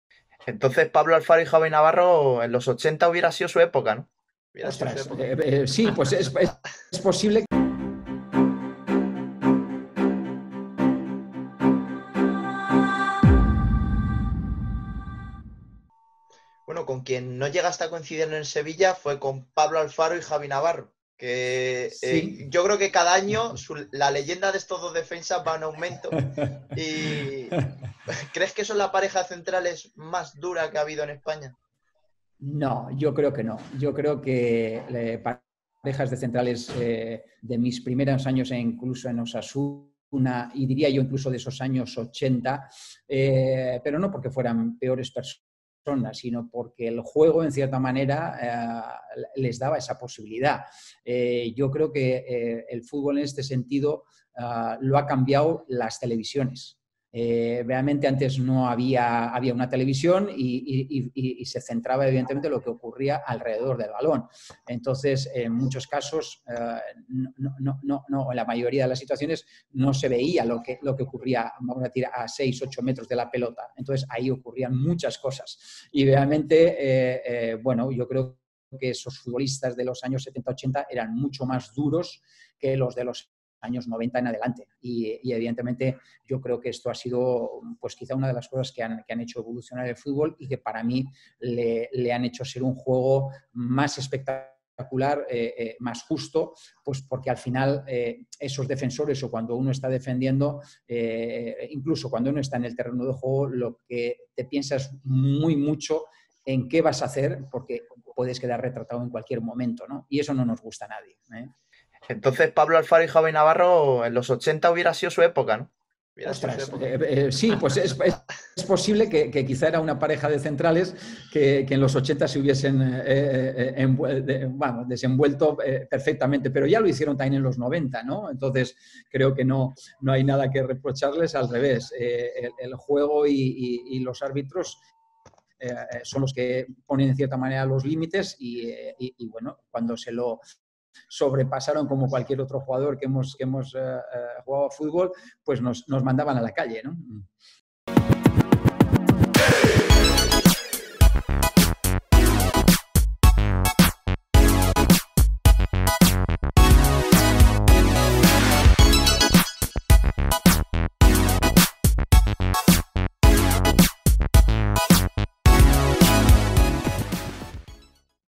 Pablo Alfaro, Javi Navarro y la defensa más dura de la historia: ¿verdad o mito?
En la entrevista a Juan Carlos Unzué sale a debate la pareja de centrales más famosa de España: Pablo Alfaro y Javi Navarro. Una pareja a la que el técnico navarro posiciona en la era de los 80 como su mejor fase para haber jugado. Y es que según el entrenador, las cámaras han hecho que ciertas acciones desaparezcan de los partidos.